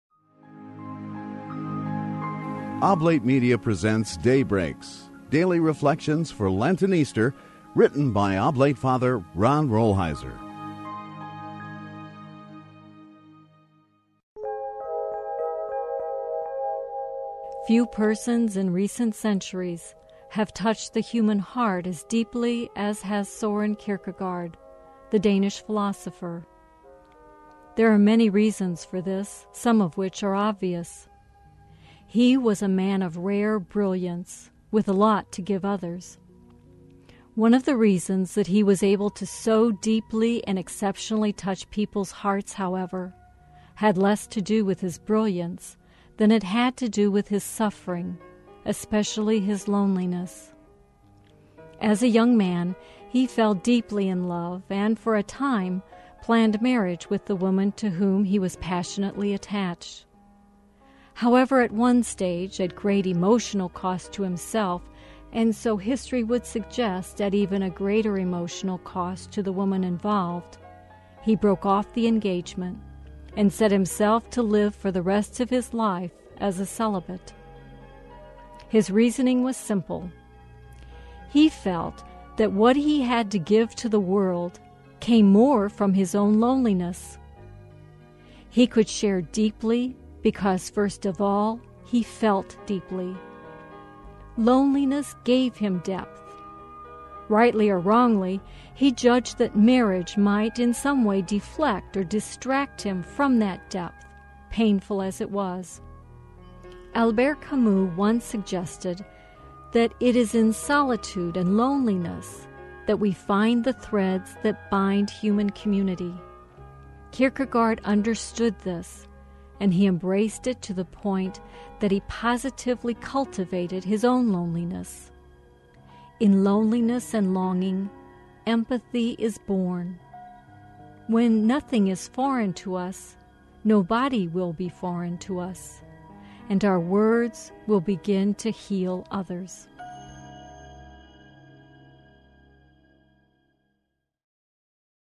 Daily Lenten Reflection for February 23, 2024 Written by Fr. Ron Rolheiser, OMI
Audio reflection 2:25 in length